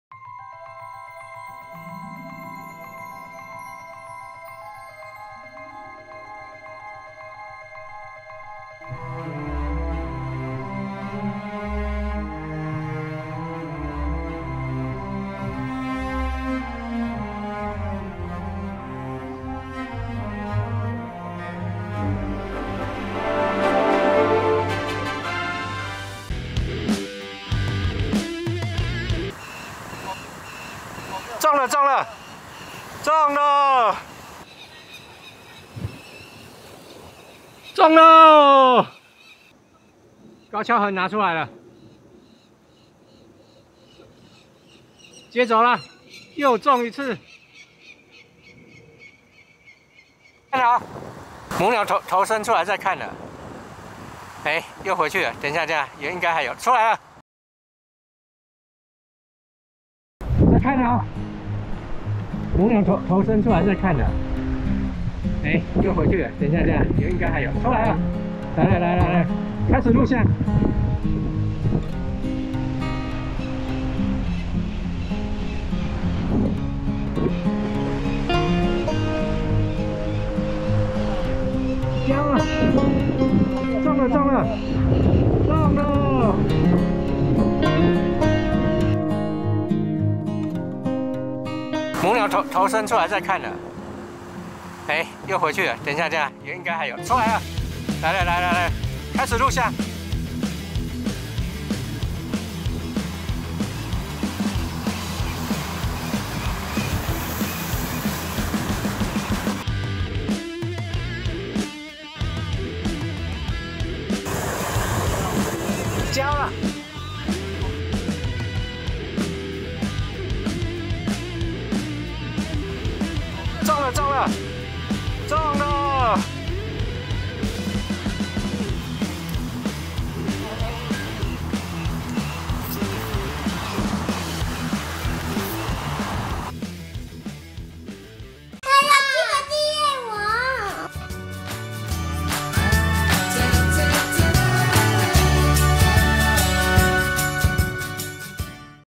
(特別版)_酋長岩遊隼_SP08_餌渡_一天三次空中交接_第一次_Falco peregrinus_2023年5月20日
2023年5月20日酋長岩遊隼餌渡_一天三次空中交接_第一次
本日MVP:高蹺鴴
這是拍攝期間特別花時間剪輯的一段影片,
由於最近太忙影片趕不及所以拿來墊墊檔,
雖然是墊檔影片,但是保證精彩XD

SONY A7S3  4K120 , SONY 200-600mm